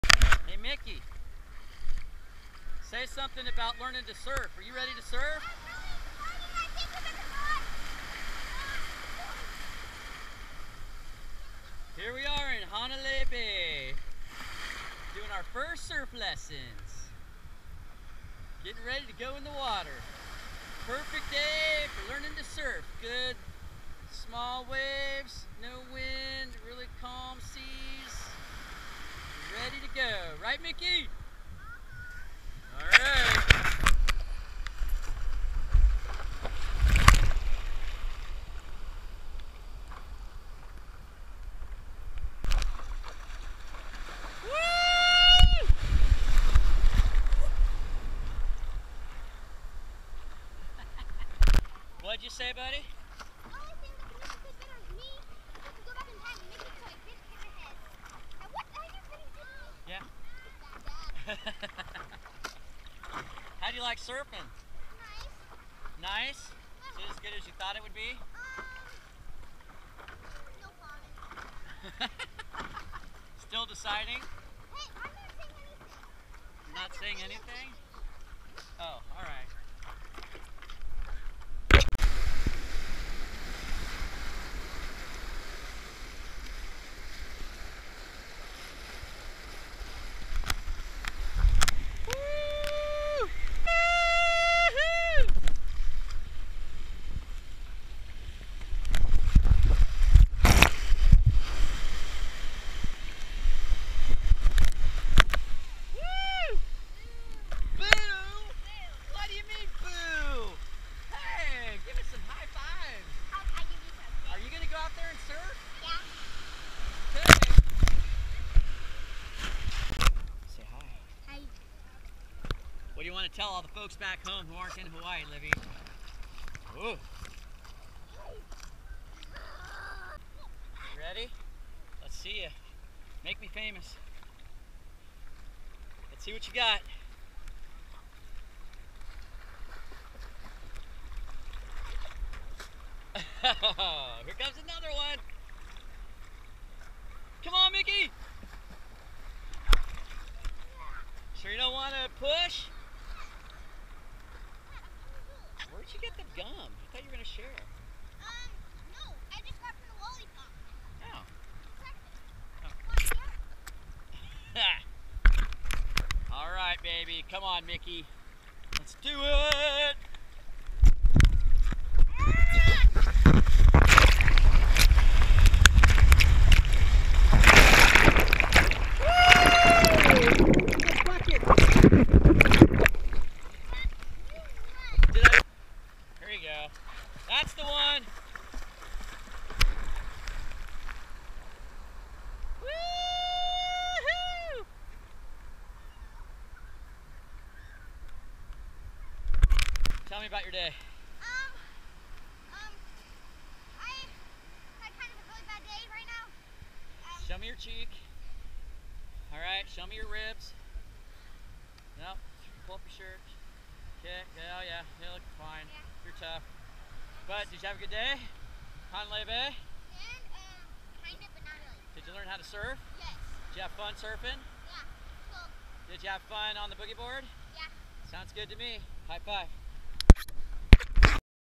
Surfing Honilei Bay
Mickey and Mike Zyskowski learning to surf on Honilei bay.  Cameos by Livy Hjort and Andy Camp.  Aloha!